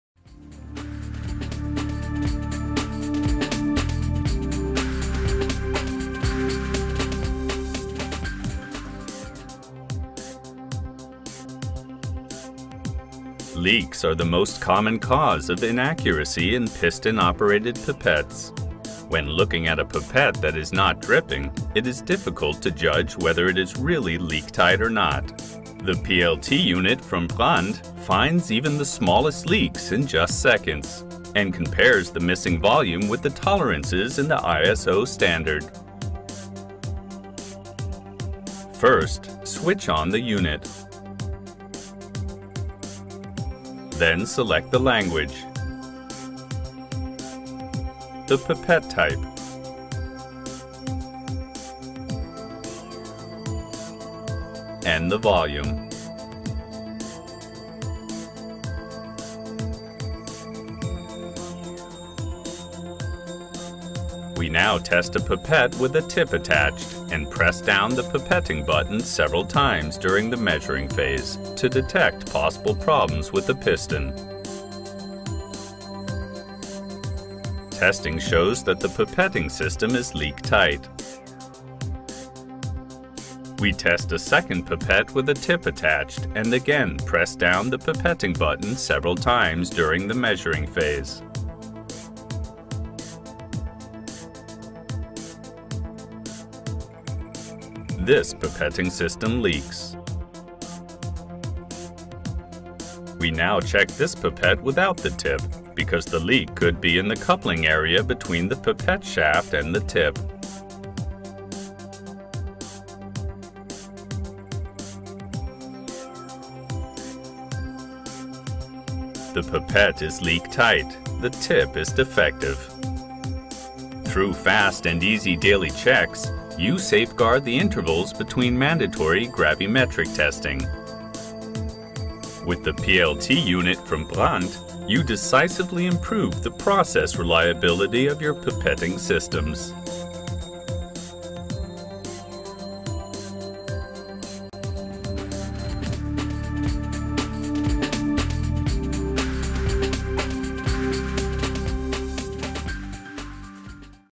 Brand Pipette Leak Testing Unit
Improve process reliability with functional validation of air-displacement pipettes!

The most frequent cause of inaccuracy in piston-operated pipettes is leakage. This arises from damage either to the seals, pistons, or tip cones. Often not detectable by the naked eye, leaks lead to significant volume errors. The BRAND pipette leak tester (PLT unit) for air displacement pipettes finds even the smallest leaks within seconds.